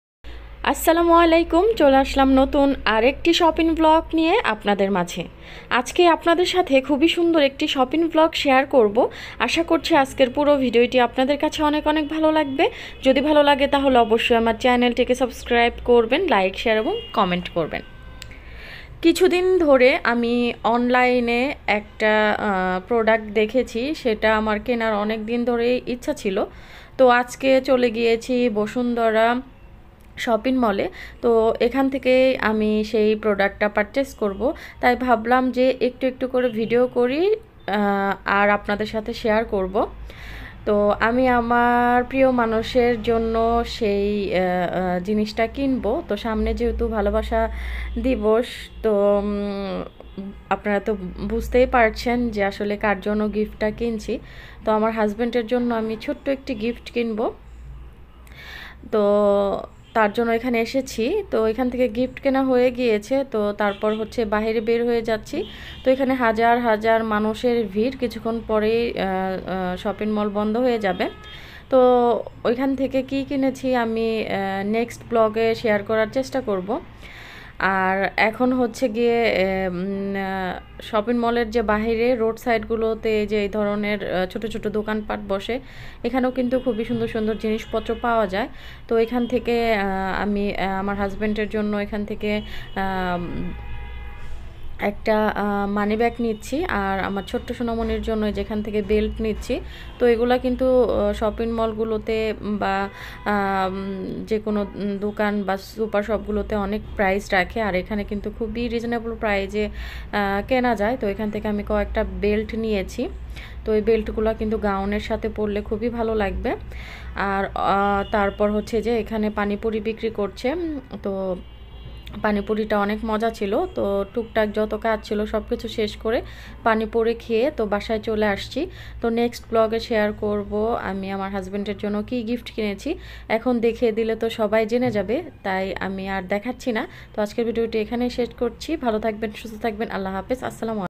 শপিং ব্লগ//অনেক কষ্টের টাকা দিয়ে গিফট কিনলাম/বসুন্ধরা শপিং মল
শপিং ব্লগ//অনেক কষ্টের টাকা দিয়ে গিফট কিনলাম/বসুন্ধরা শপিং মল ঢাকা